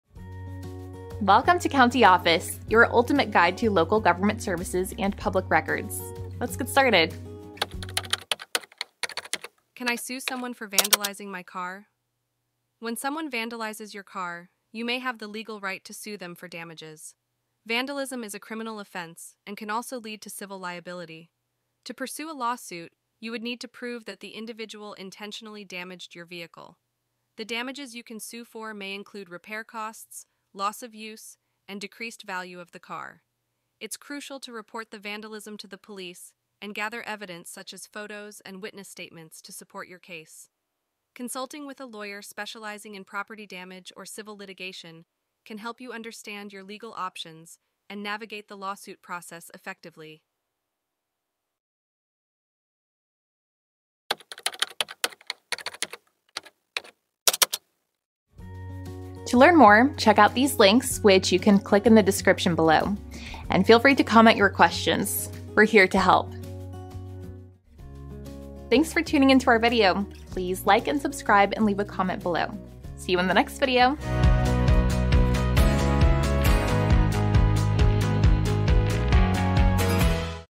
Can I Sue Someone For Vandalizing My Car? - CountyOffice.org
Can I Sue Someone For Vandalizing My Car? In this video, we explore the legal aspects of suing someone for vandalizing your car. Vandalism is not only a criminal offense but can also result in civil liability. To pursue a lawsuit in such cases, you must be able to demonstrate that the damage was intentional. Potential damages you can seek in a lawsuit may include repair costs, loss of use, and the diminished value of your vehicle. It is crucial to report the vandalism to the police and gather evidence such as photos and witness statements to strengthen your case. Consultation with a lawyer specializing in property damage or civil litigation can provide you with valuable insights into your legal options and help you navigate the lawsuit process effectively.